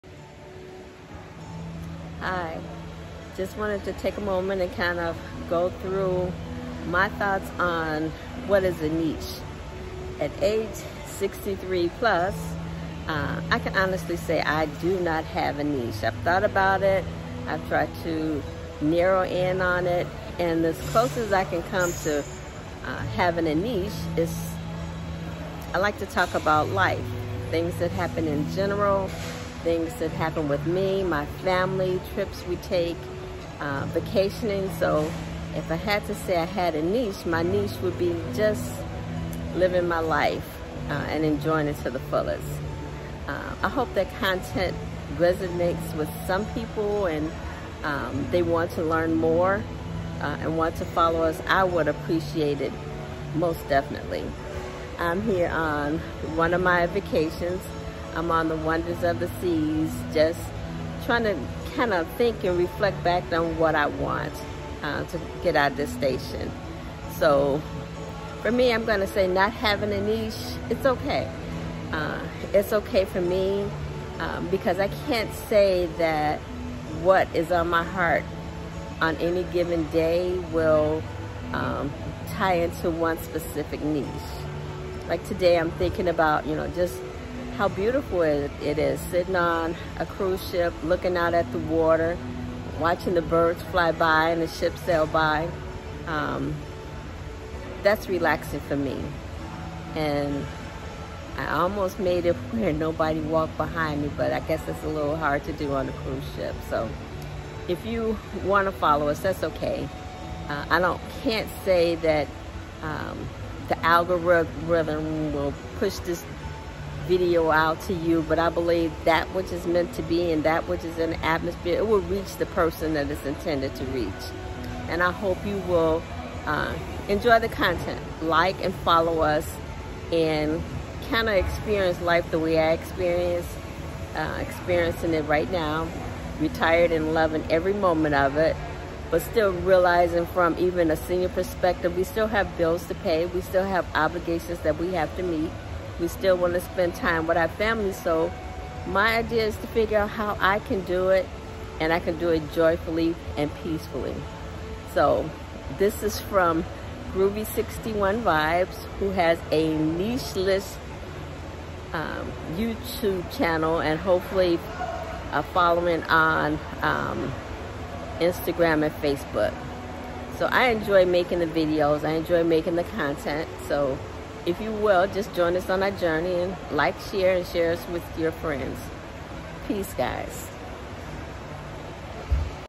DO I Need a Niche? My content crosses niches!
Trying to identify One niche has been hard! I don’t know if youtube will push my content out but trying to focus on one topic is not what my life has been about! life lessons positivefaith positiveaffirmations travelvlog cruise @groovy61vibes